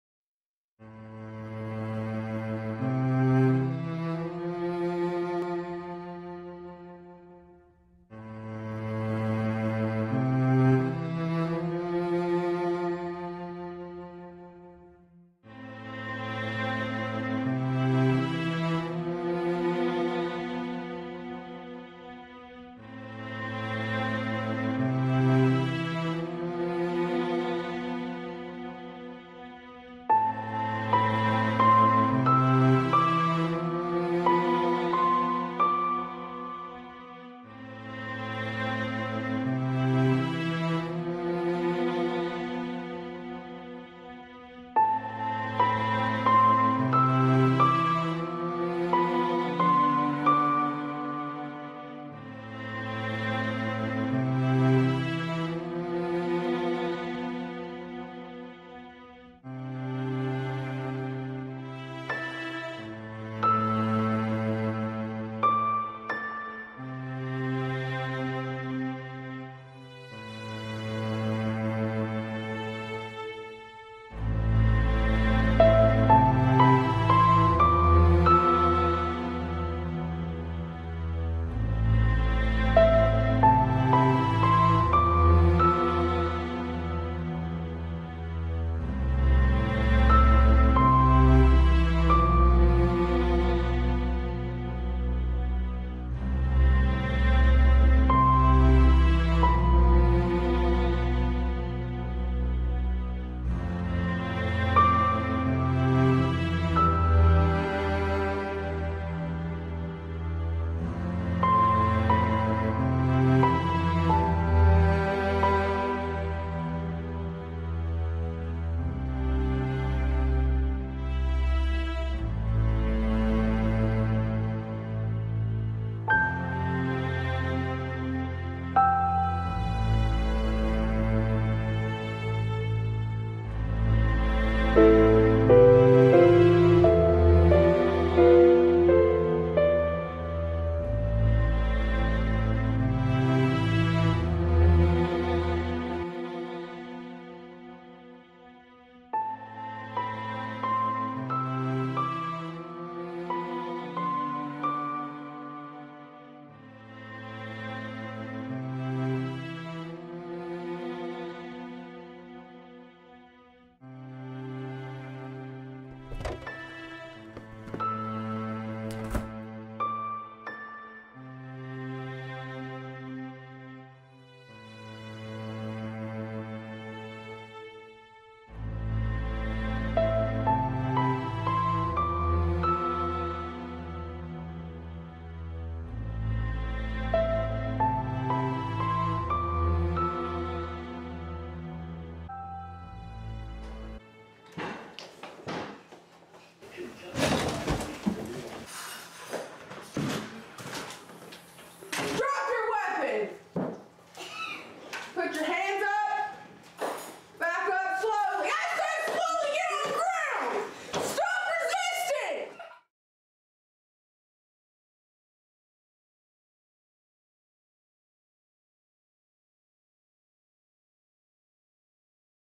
Home Sweet Home (Short-Film)
A short film about a home invasion, and a failed attempt at a second one. (Freshman-Project)